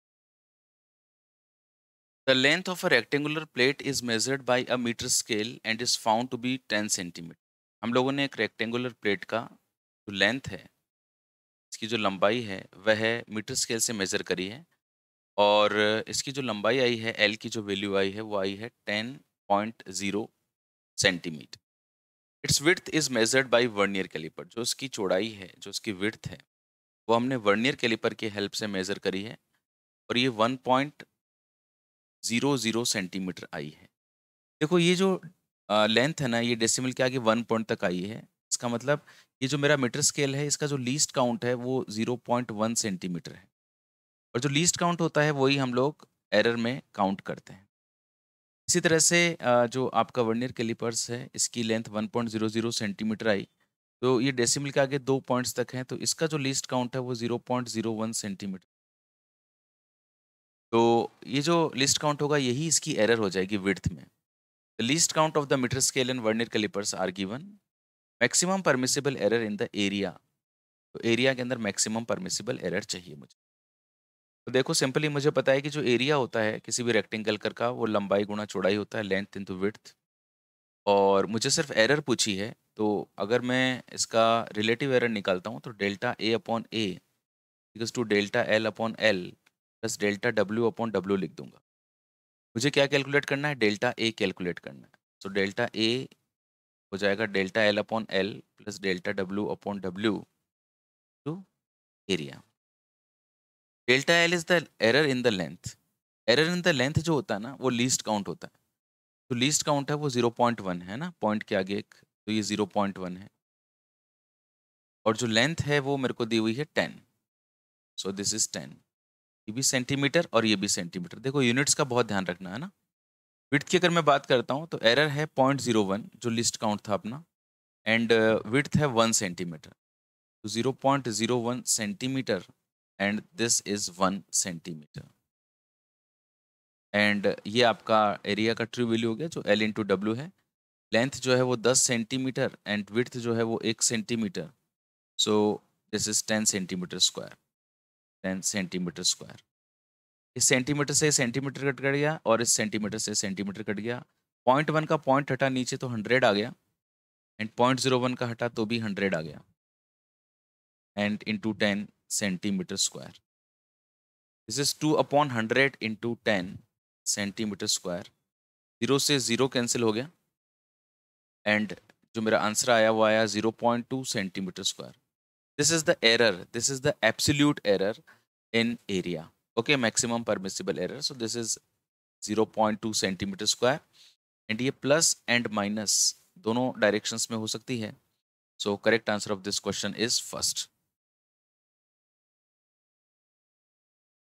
The length of a rectangular plate is measured by a meter scale and is found to be 10.0 cm. Its width
The length of a rectangular plate is measured by a meter scale and is found to be 10.0 cm. Its width as 1.00 cm. The least count of the meter scale and Vernier callipers are 0.1 cm and 0.01 cm respectively. Maximum permissible error in area is measured by Vernier callipers measured is